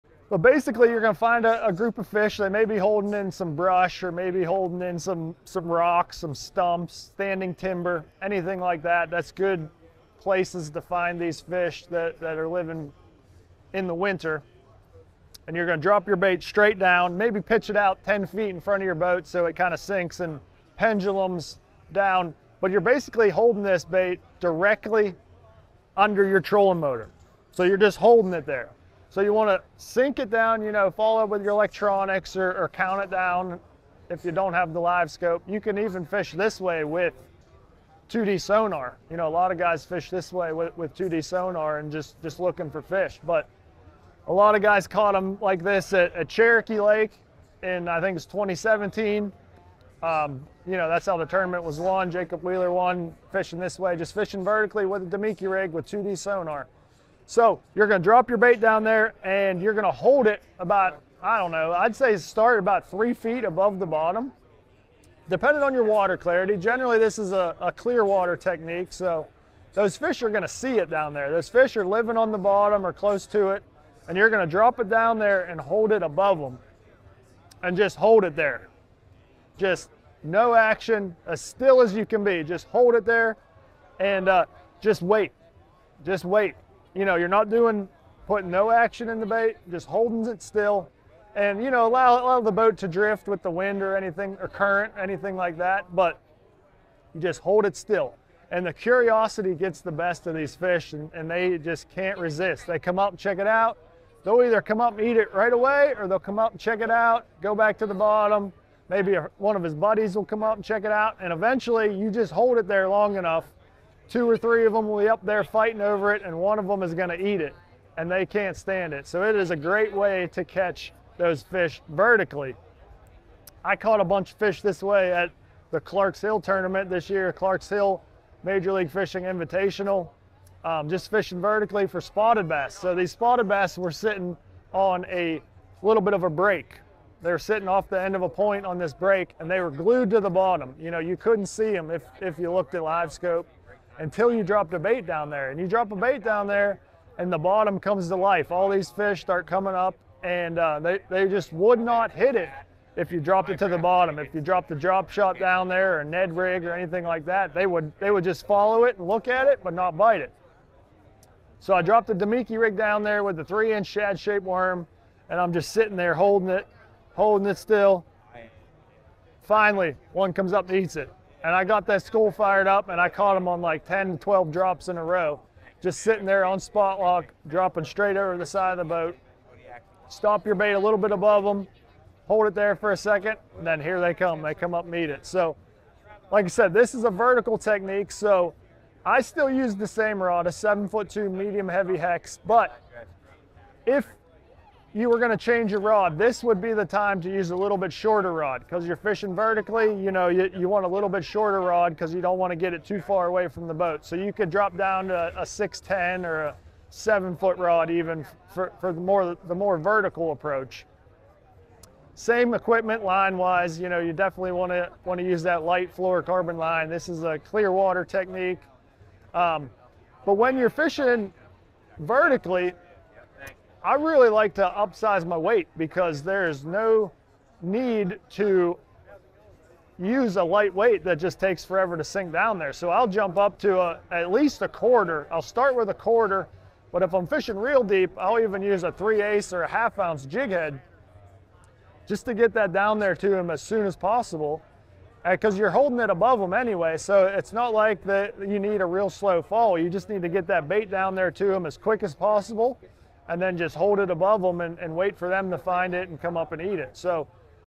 Vertical Bass Fishing Tips - SECRETS Pros Don't Want You to Know
Unlock the secrets of vertical finesse fishing for bass with a MLF Bass Pro Tour Angler of the Year Matt Becker. Matt shares his proven technique of dropping bait directly under the boat, enticing winter bass in brush, rocks, and standing timber. Discover the art of holding still, enticing curious fish to strike. Great finesse bass fishing tournament technique.